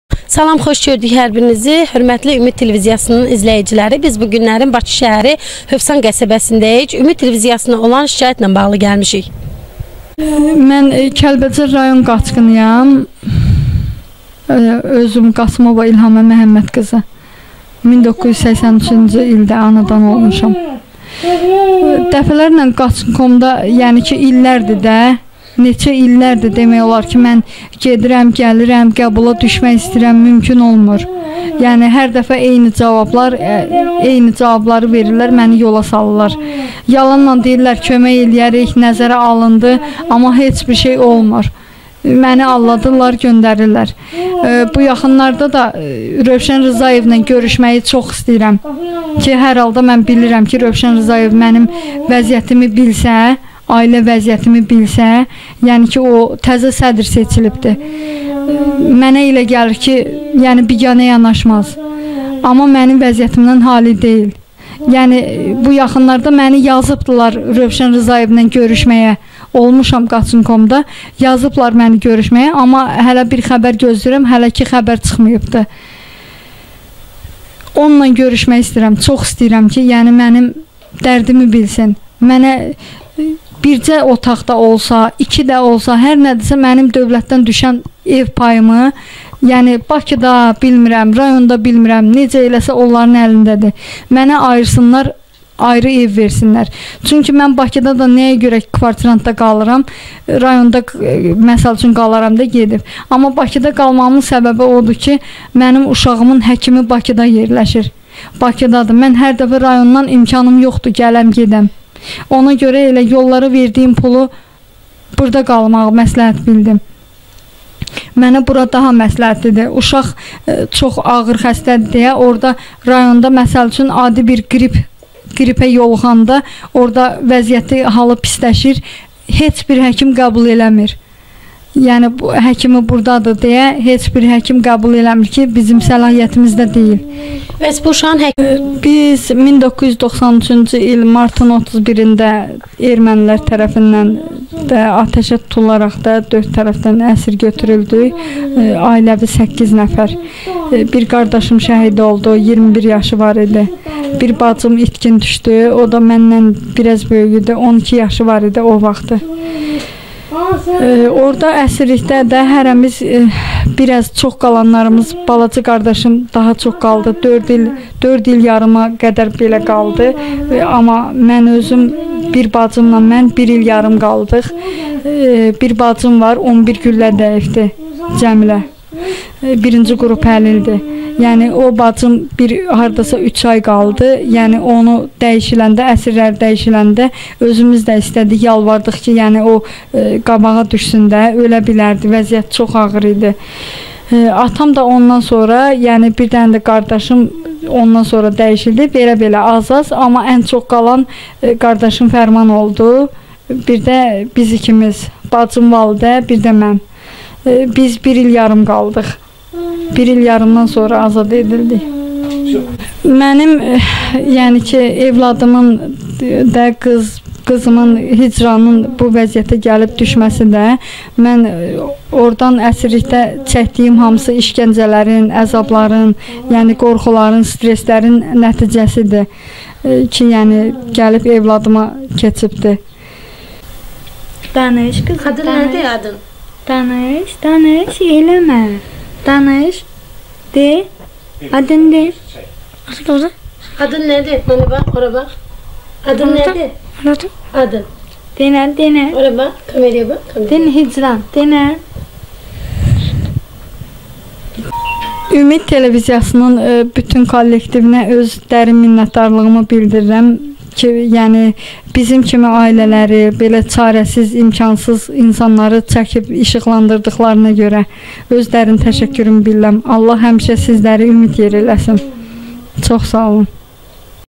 HÖVSANDA AĞIR ŞƏRAİTDƏ YAŞAYAN ANA XƏSTƏ QIZINA KÖMƏK İSTƏYİR
KANALA ABUNƏ OLMAĞI UNUTMA !
ŞAHİDİ OLDUĞUNUZ HADİSƏLƏRİ BİZƏ GÖNDƏRİN.